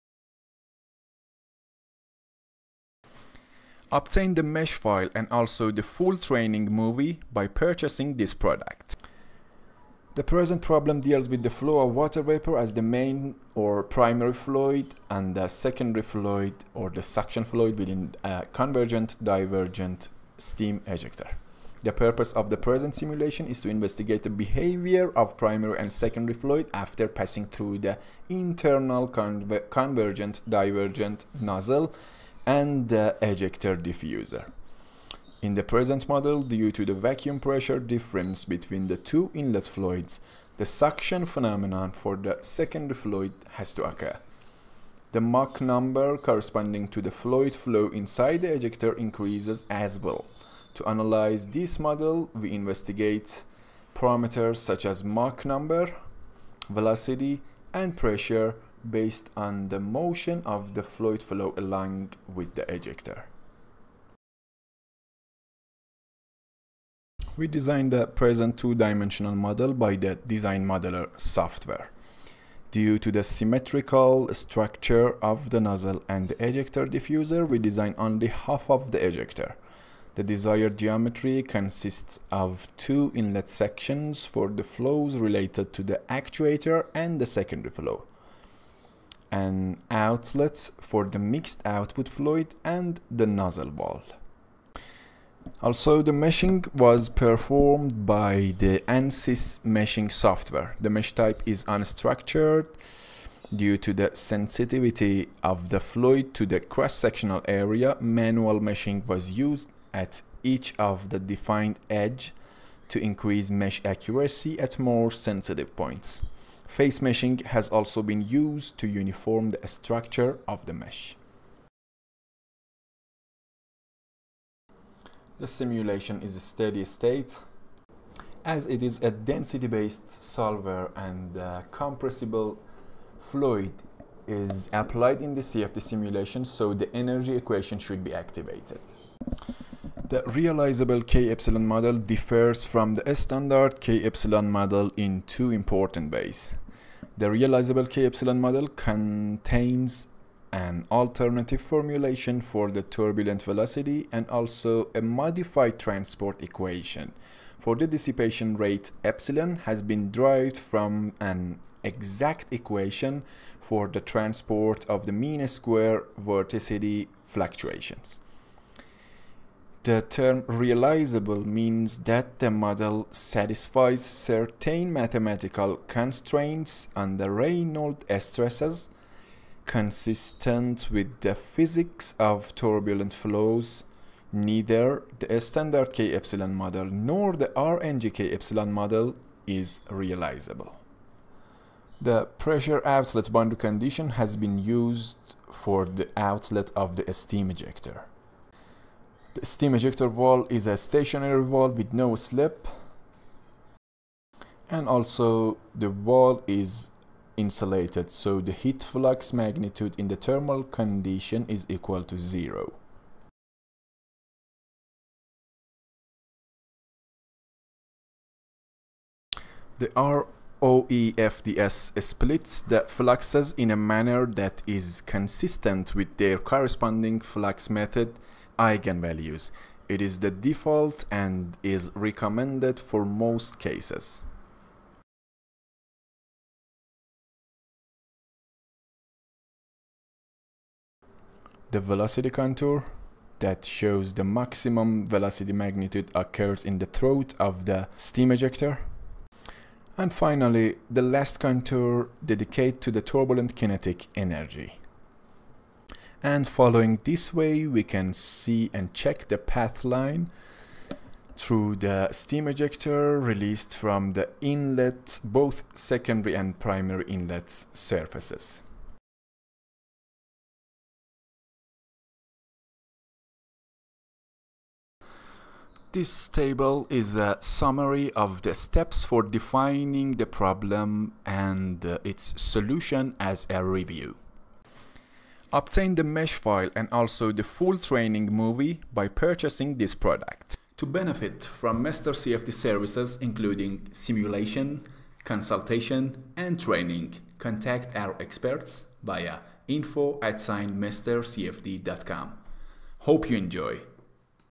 Steam Ejector
Ejector CFD Simulation, Two-Phase Flow

The present problem deals with the flow of water vapor as the main fluid (primary) and the secondary fluid (suction) within a convergent-divergent ejector. The purpose of the present simulation is to investigate the behavior of primary and secondary fluid after passing through the internal convergent-divergent nozzle and the ejector diffuser. In the present model, due to the vacuum pressure difference between the two inlet fluids, the suction phenomenon for the secondary fluid has to occur. The Mach number corresponding to the fluid flow inside the ejector increases as well. To analyze this model, we investigate parameters such as Mach number, velocity, and pressure based on the motion of the fluid flow along with the ejector.